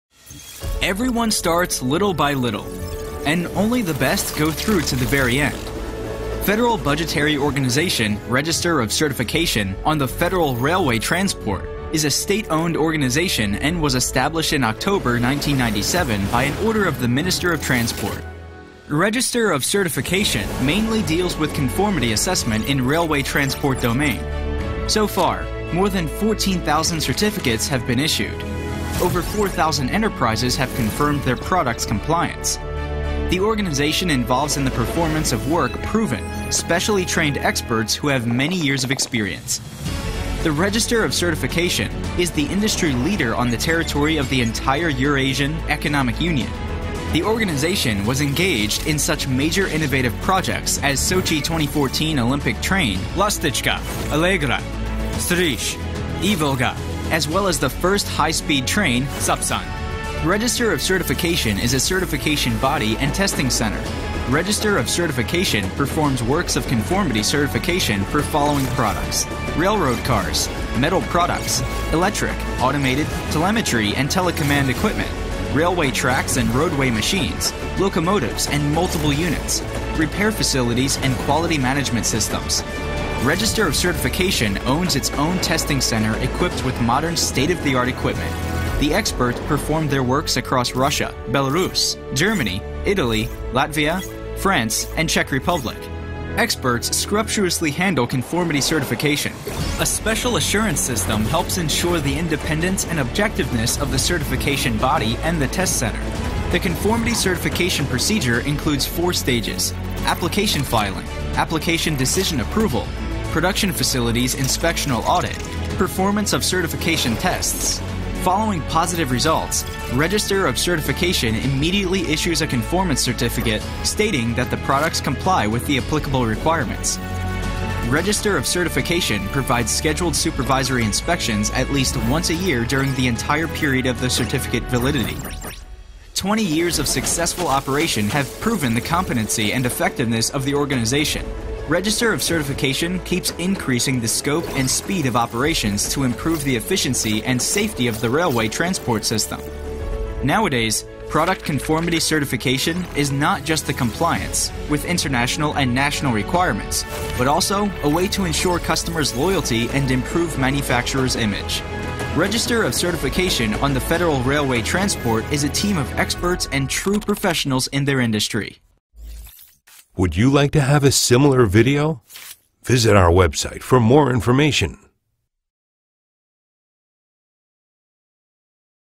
3D Company Presentation Video. Corporate 3D Video Production Services. Railway Company 3D Animation
In the age of the totally domination of video content in the internet, the best way to present your busines or product to the potential customers or partner is to create a company presentation video. This video for the russian government owned certification company allows to explain the purpose of complex products in simple words in just a few minutes. 3d-modelling allows to attract and hold the viewer's attention of the viewers for the whole lenght of the presentation.